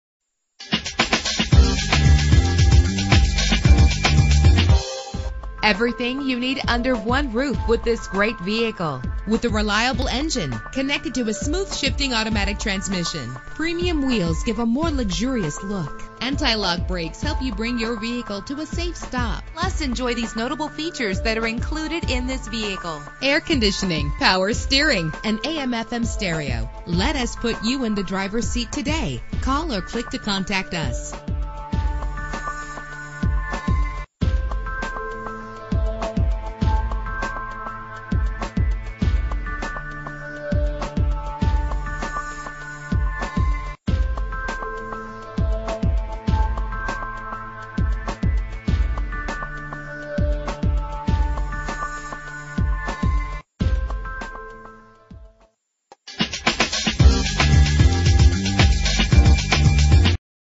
2000 Chevrolet 3500 Crew Cab - Whitefish MT
Year: 2000
Make: Chevrolet
Model: 3500 Crew Cab
Trim: Long Bed
Engine: 8 cylinder Turbo Diesel 6.5 liter
Transmission:
Color: White
Mileage: 42590
Address:
6331 HWY 93 South